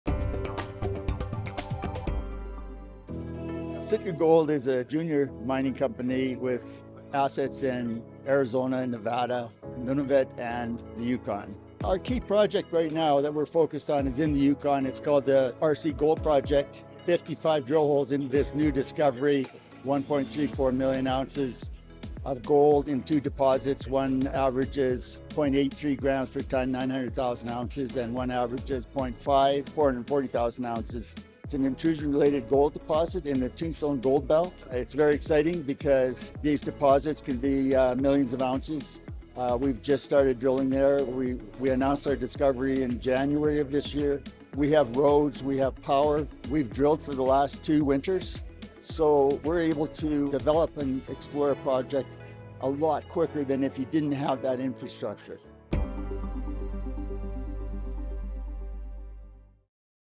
Sitka Gold & Their Exciting Journey in the RC Gold Project, Yukon
Investing in Gold

Sitka Gold, a junior mining company, has assets in Arizona, Nevada, Nunavut, and the Yukon, with their primary focus on the RC Gold Project in the Yukon. This project has revealed 1,340,000 ounces of gold across two deposits through 55 drill holes. The first deposit contains 900,000 ounces of gold at an average of 0.83 grams per tonne, while the second has 440,000 ounces at 0.5 grams per tonne. Located in the Tombstone Gold Belt, these are intrusion-related gold deposits with the potential to yield millions of ounces. Drilling at this site began recently, with the discovery announced in January of this year. The project benefits from existing infrastructure like roads and power, enabling faster development and exploration than in areas without such facilities. Drilling has been ongoing for the past two winters.